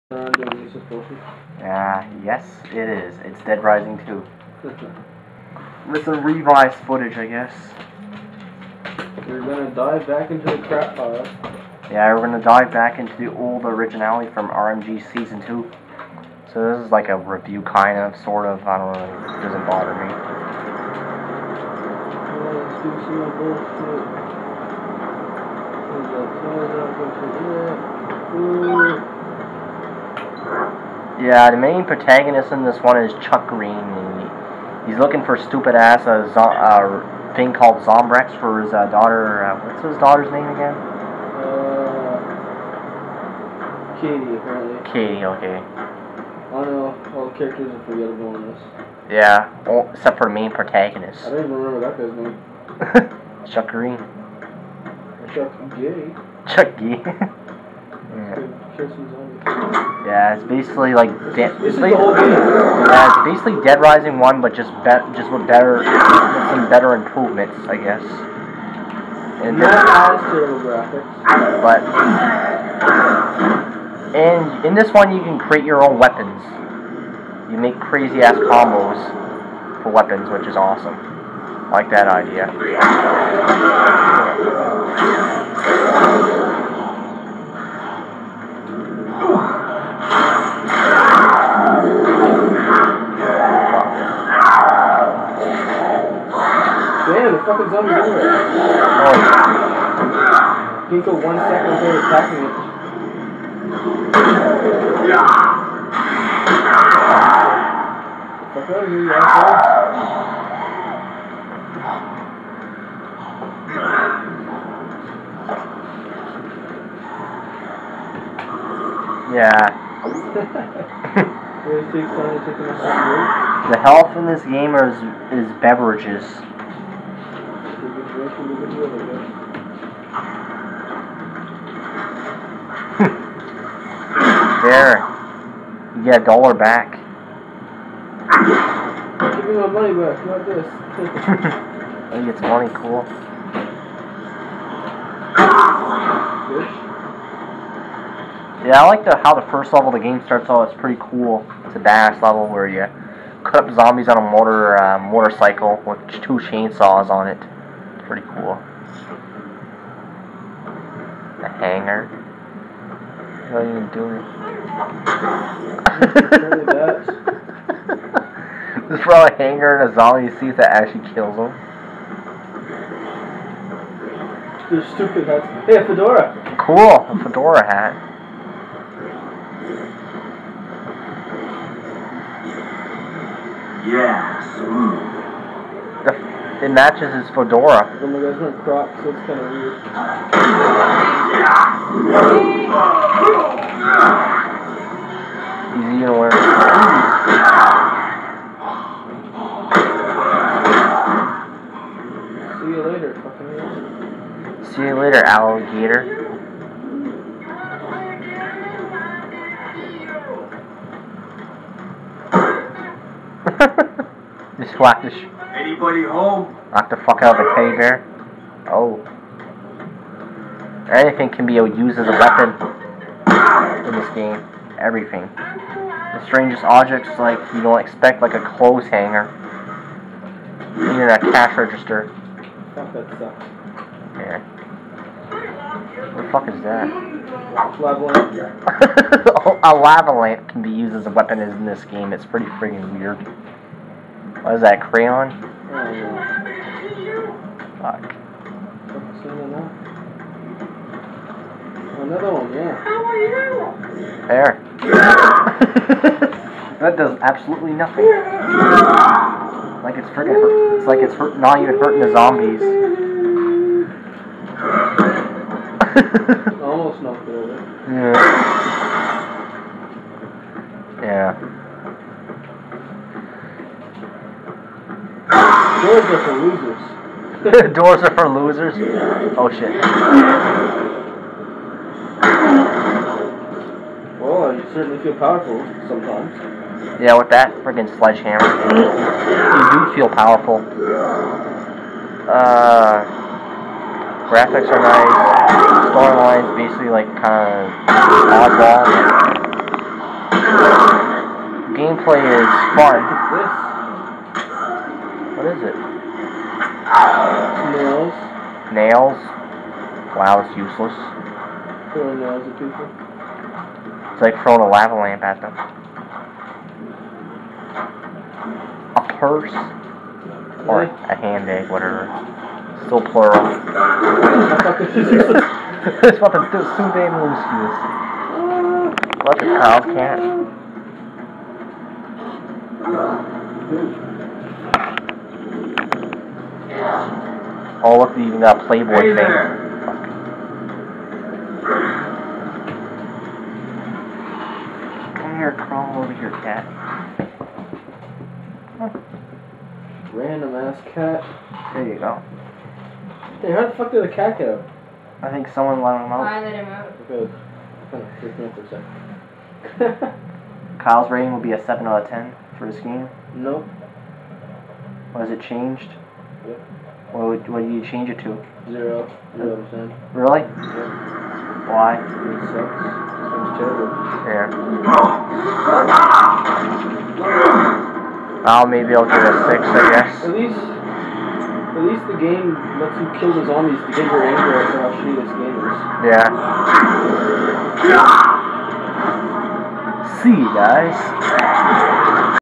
dead rising 2 revisited game review
Killer J and Phantom
Review Dead Rising 2 on Xbox 360
RMG Classic